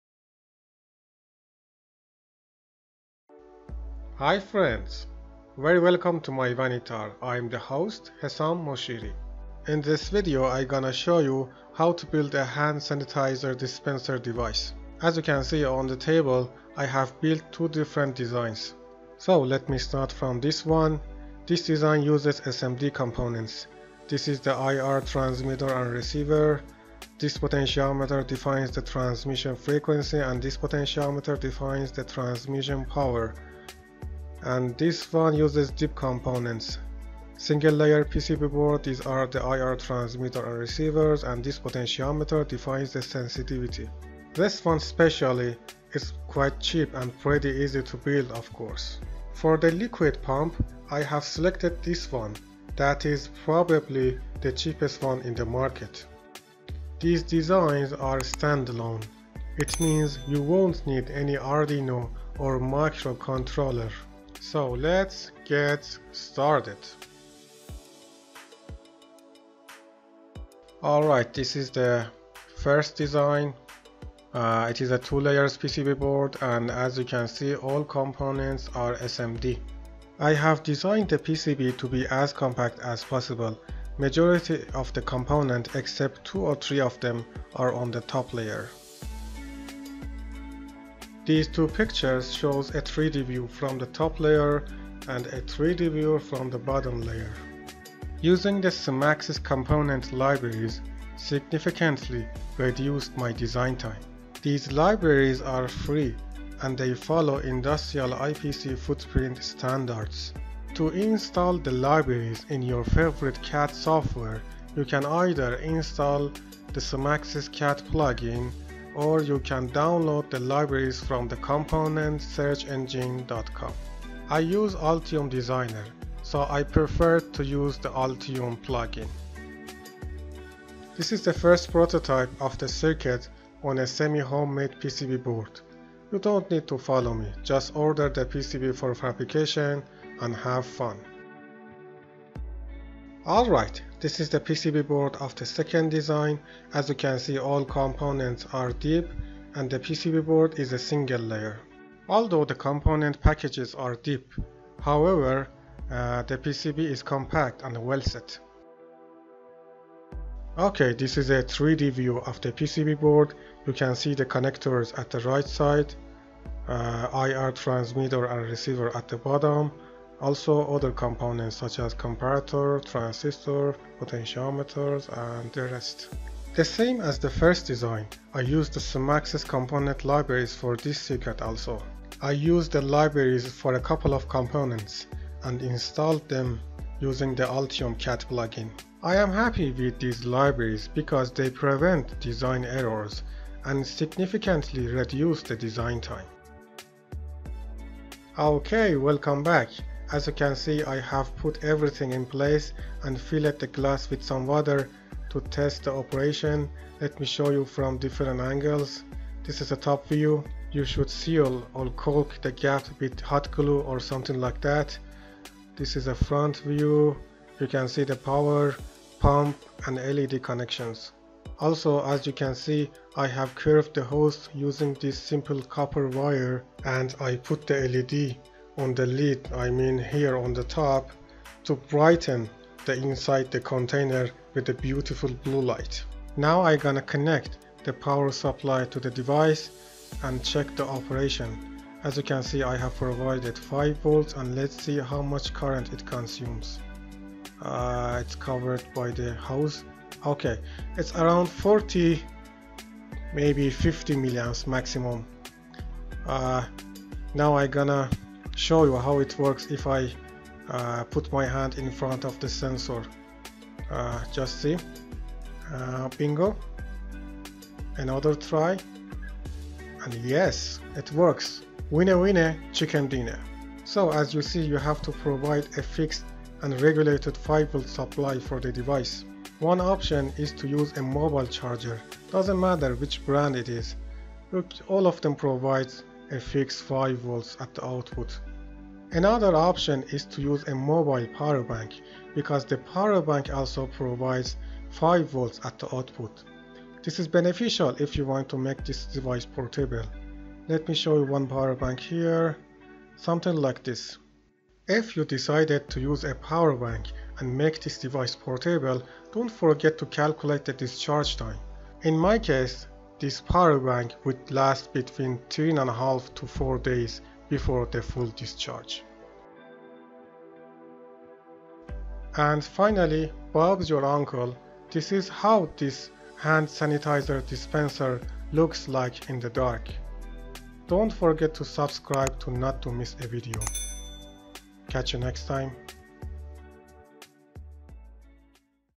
Hand Sanitizer/Alcohol Dispenser Circuit/DIY, Without an Arduino/MCU [Fight with COVID-19]
DIY Automatic Hand Sanitizer/Alcohol Dispenser [Non-Contact], without an Arduino/MCU [Fight with COVID-19]

In this article/video, you will learn how to build a hand sanitizer/alcohol dispenser device, without any MCU, programming, or Arduino. let's fight with COVID-19 and COVID-20 with technology.

Inspired by:
TechBuilder
Inspired by:
NONC
Inspired by:
Global Fun